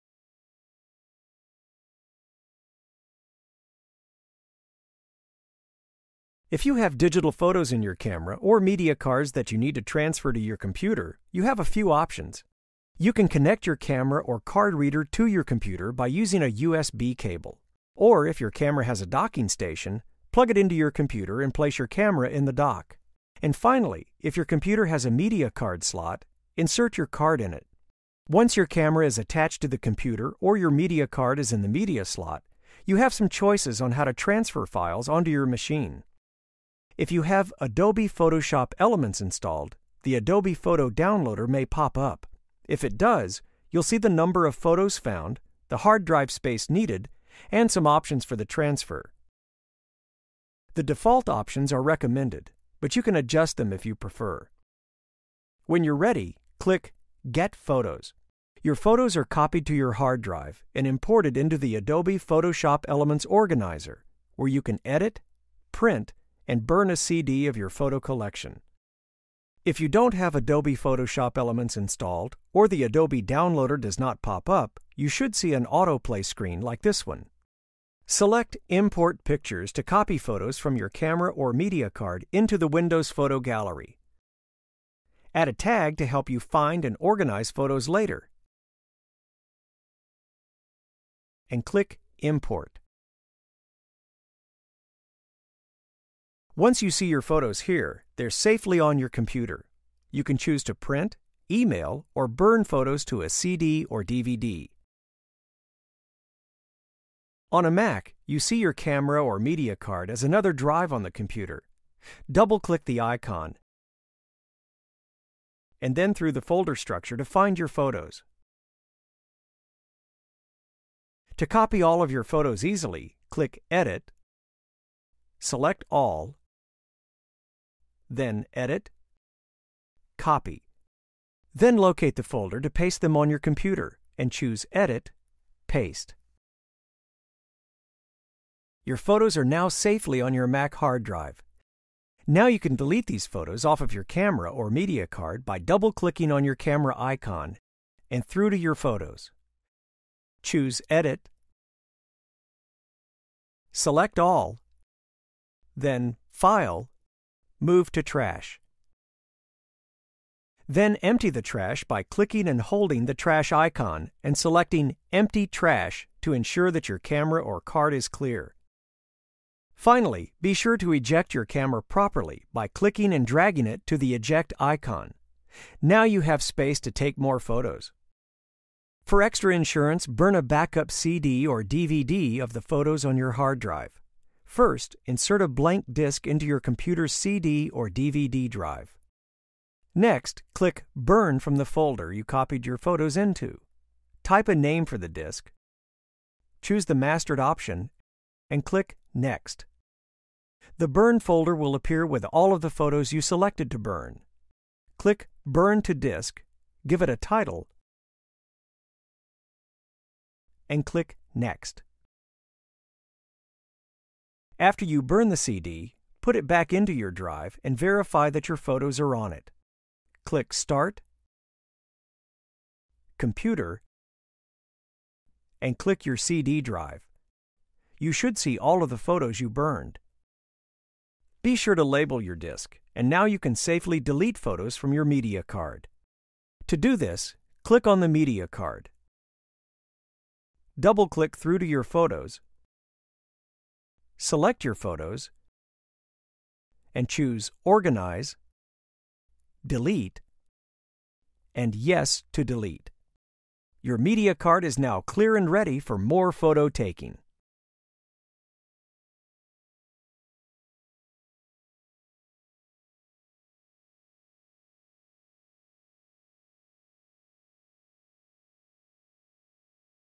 How to Transfer Photos from Your Digital Camera to Computer For Dummies
Transferring digital photos from your camera is necessary to clear your memory card. Adobe Photoshop Elements makes downloading photographs easy, or you can import pictures to your computer with Windows. Then you can make prints, copy photos to a CD or DVD, and share them online.